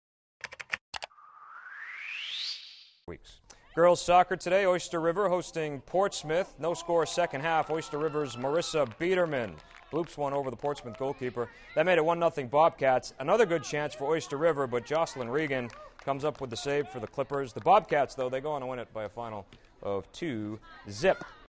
Monday Girls Soccer
Oyster River able to hold off Portsmouth.